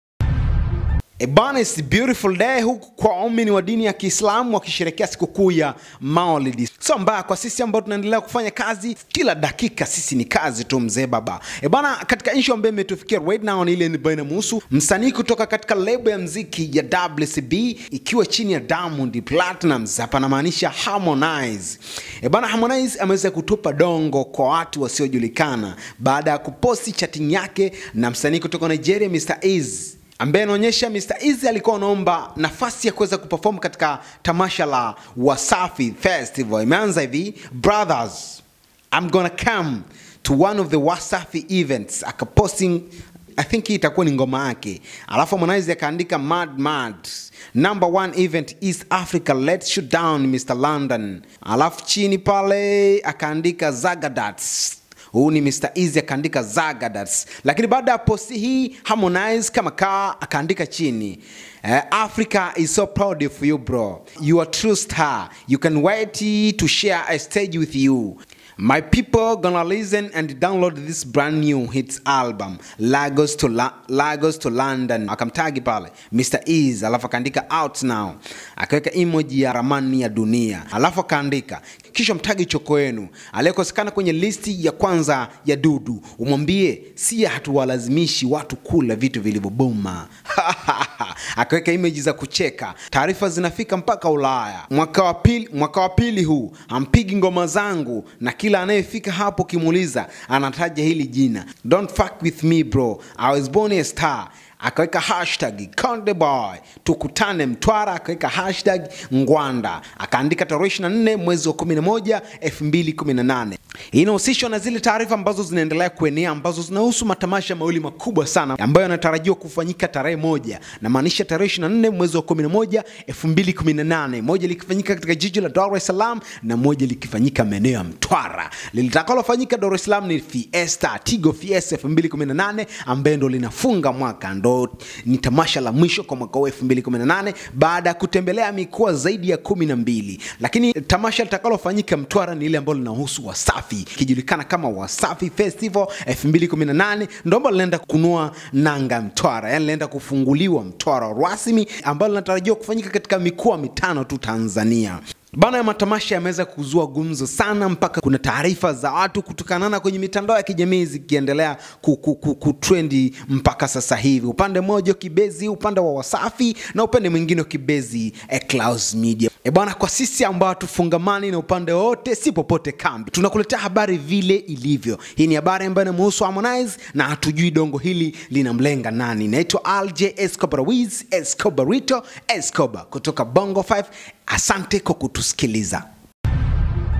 HARMONIZE ATOA YA MOYONI/ATOBOA SIRI/HAMCHEZI NYIMBO ZANGU MWAKA WA PILI SASA
HARMONIZE ATOA YA MOYONI/ATOBOA SIRI/HAMCHEZI NYIMBO ZANGU